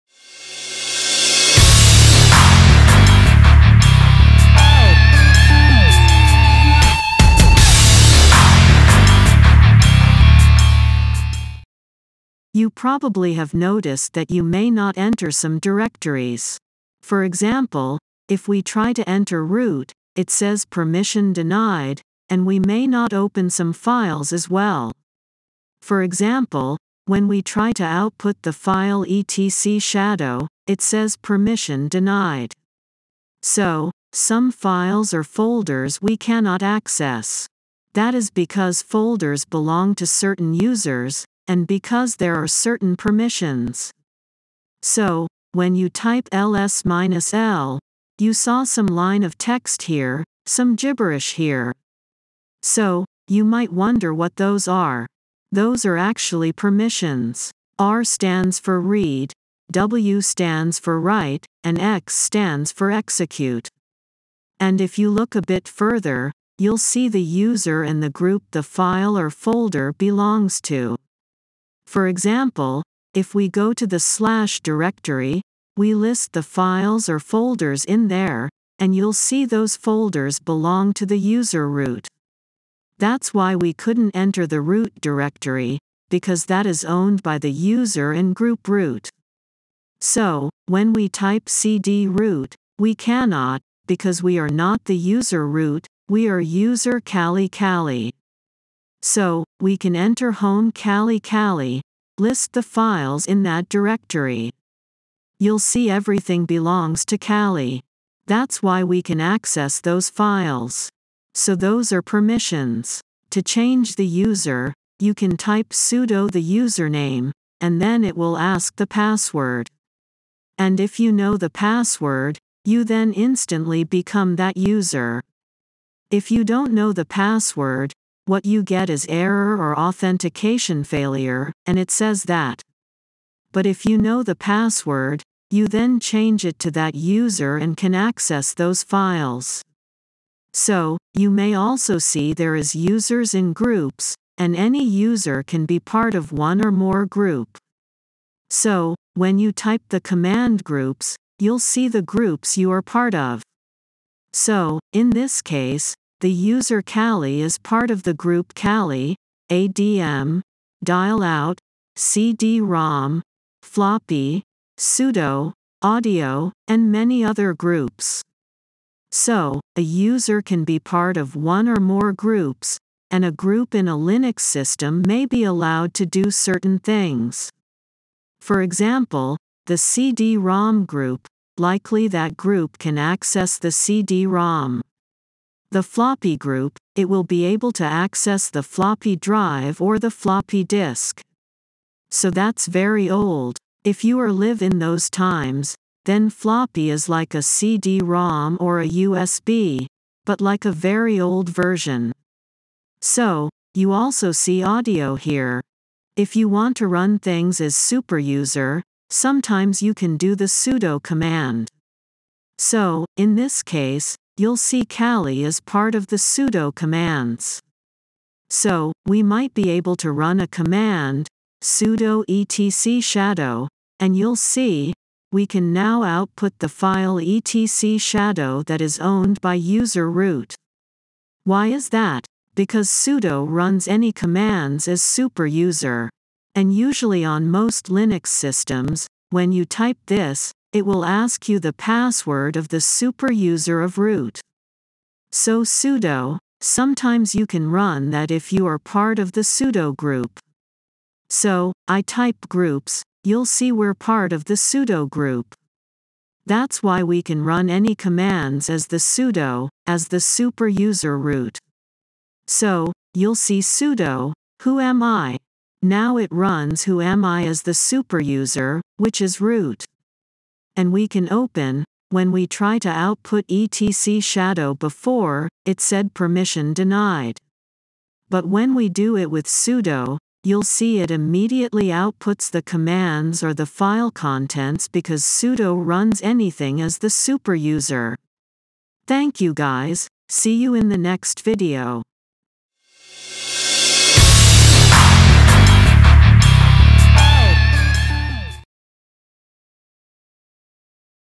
Linux Command line for Beginners 08 | Linux Terminal | Permissions
Welcome to the 8th episode of our "Linux Command Line for Beginners" series! In this video, we focus on file permissions in the Linux Terminal.

Join us as we explore the importance of file permissions and learn how to effectively manage them. Understanding and properly configuring file permissions is crucial for maintaining security and controlling access to files and directories in Linux.

✅ Permission Basics: Gain a solid understanding of the different permission types and their significance in Linux.
✅ Changing Permissions: Learn how to modify file permissions using symbolic notation or octal values.
✅ Special Permissions: Explore special permissions such as setuid, setgid, and sticky bit and their uses.
✅ Permission Masks: Discover how permission masks simplify the process of assigning permissions to multiple users or groups.

In this tutorial, we provide practical examples and demonstrate various command line techniques to work with file permissions. By the end of the video, you'll be equipped with the knowledge and skills to effectively manage permissions and enhance the security of your Linux system.

Whether you're a beginner delving into the Linux command line or an experienced user seeking a deeper understanding of file permissions, this tutorial is designed to empower you in managing access and security in Linux.